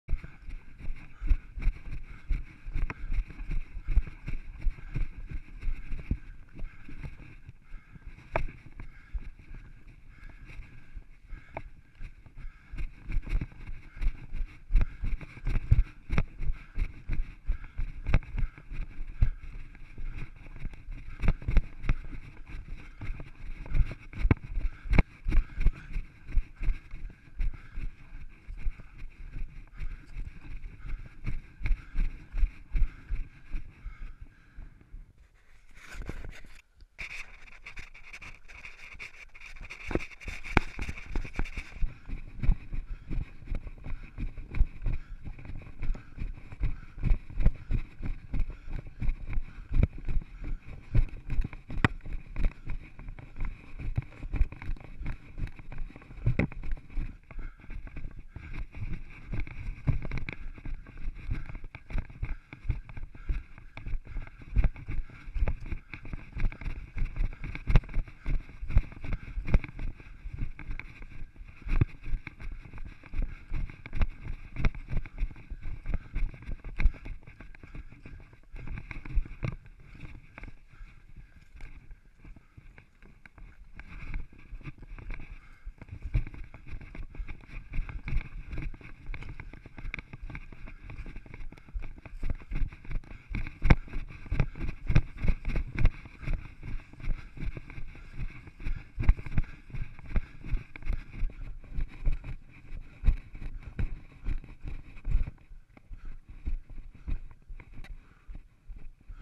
Wilderness Running
When there's no trails, you gotta make them up: playing around Bay de Verde Head.